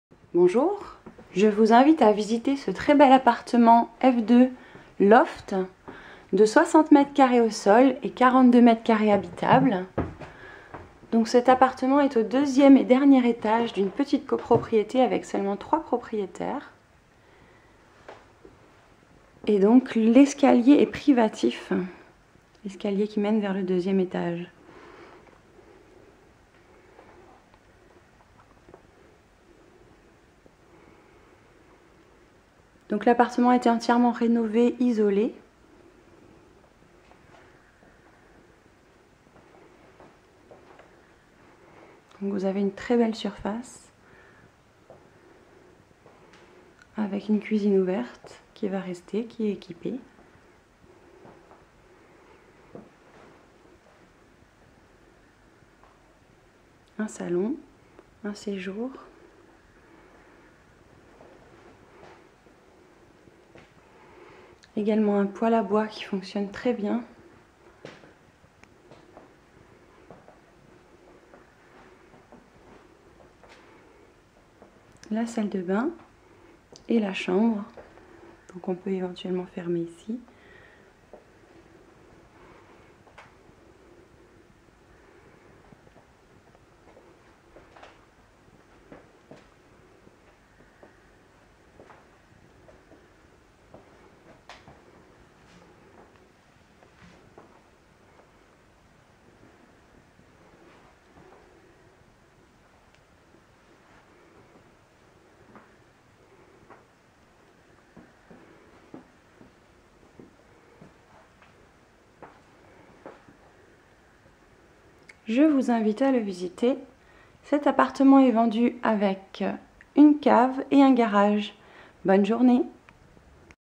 Loft 2P - A vendre - 67000 STRASBOURG - 159 000 €
Vous serez séduits par ce magnifique F2 – LOFT de 60m² au sol (42m² carrez), avec le cachet des poutres apparentes. Ce beau LOFT est situé à STRASBOURG La Robertsau, (Rue du Chevalier Robert), au 2ème et dernier étage sans ascenseur, et à proximité des commerces et des transports en commun.

- AGENCE :
Robin des Toits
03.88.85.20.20

- ANNONCE :
Type : Appartement Loft
Surface : 60 m2
Nb de pièce : 2